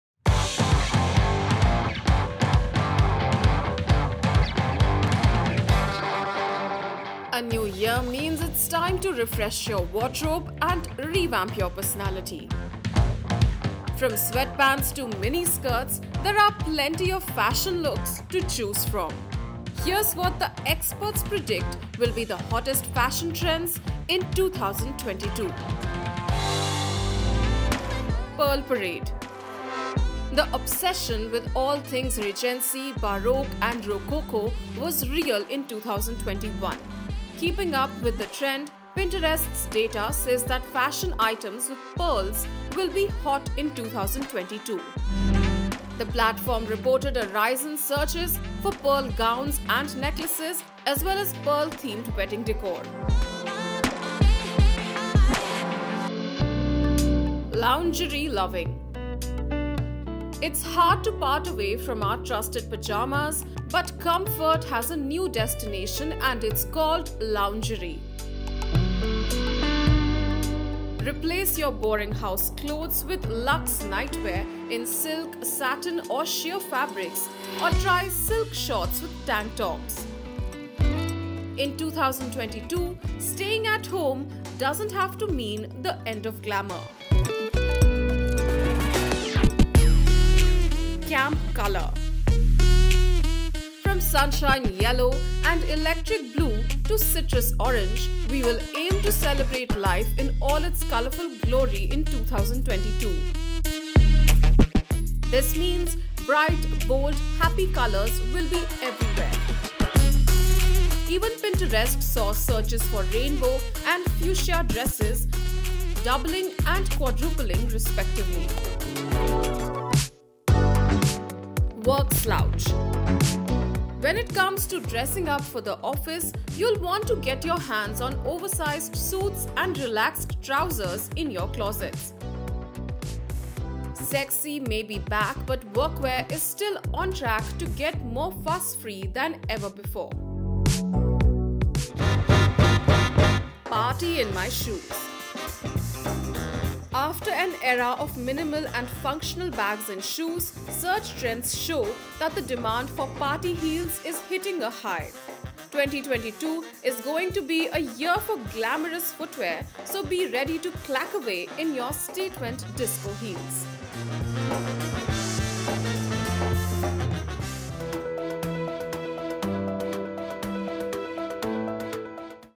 What’s in, what’s out: fashion trends that will be hot in 2022
A new year means time to refresh your wardrobe and revamp your personality. From sweatpants to miniskirts, there are plenty of fashion looks to choose from.

Here’s what the experts predict will be the hottest fashion trends in 2022.
Pearl Parade
The obsession with all things Regency, Baroque and Rococo was real in 2021. Keeping up with the trend, Pinterest’s data says that fashion items with pearls will be hot in 2022. The platform reported a rise in searches for pearl gowns and necklaces as well as pearl-themed wedding décor.

Loung-erie Lovin’
It’s hard to part away from our trusted pyjamas but the comfort has a new destination and it's called loung-erie. Replace your boring house clothes with luxe nightwear in silk, satin or sheer fabrics or try silk shorts with tank tops. In 2022, staying at home doesn’t have to mean the end of glamour. Camp Colour
From sunshine yellow and electric blue to citrus orange, we will aim to celebrate life in all its colourful glory in 2022. This means bright, bold, happy colours will be everywhere. Even Pinterest saw searches for rainbow and fuchsia dresses doubling and quadrupling respectively.

Work Slouch
When it comes to dressing up for the office, you'll want to get your hands on oversized suits and relaxed trousers in your closet. Sexy may be back but workwear is still on track to get more fuss-free than ever before.

Party in My Shoes
After an era of minimal and functional bags and shoes, search trends show that the demand for party heels is hitting a high. 2022 is going to be a year for glamourous footwear so be ready to clack away in your statement disco heels.